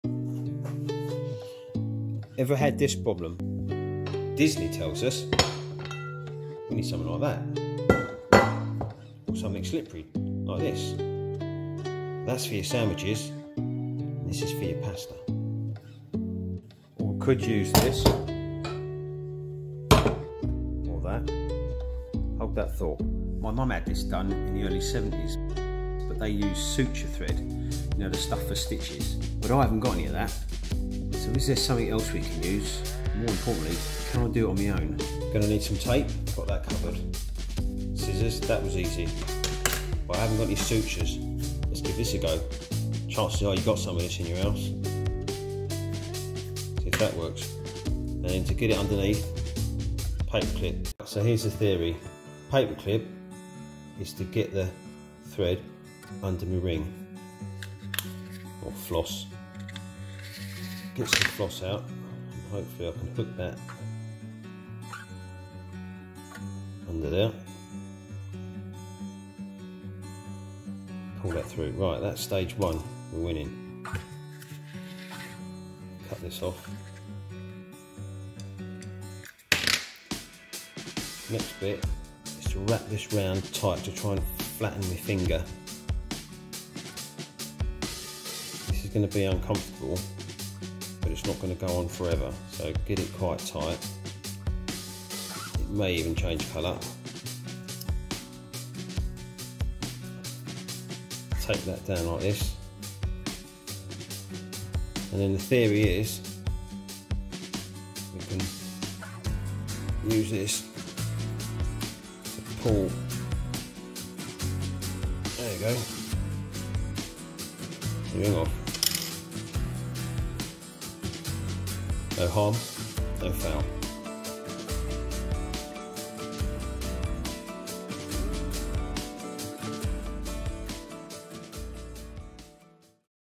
How to Remove Stuck Rings from swollen fingers!
Remove a stuck ring from a finger with some tape and a length of dental floss.